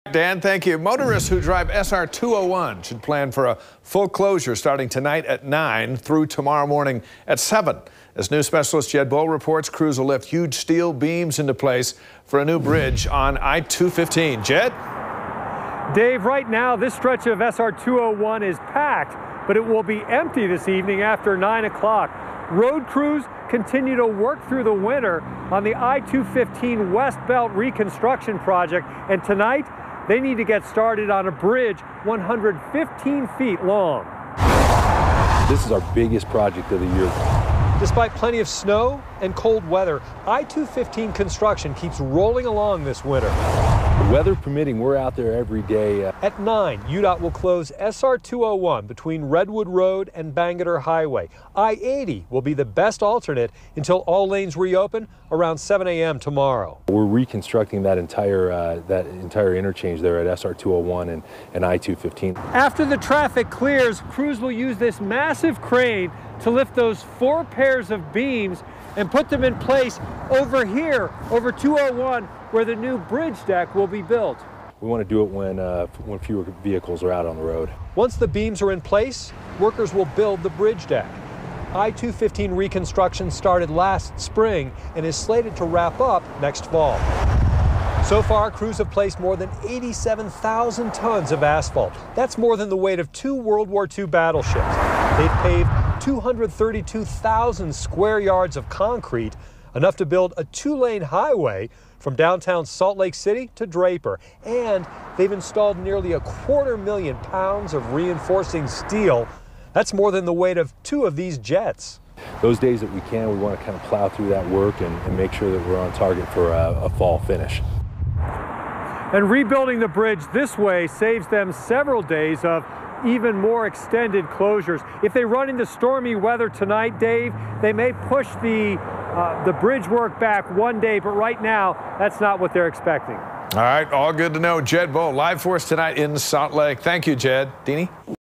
SR 201 closed while crews lift steal beams into place for new bridge on I-215
SR 201 was closed while crews lifted steal beams into place for a new bridge on I-215. News Specialist Jed Boal reports.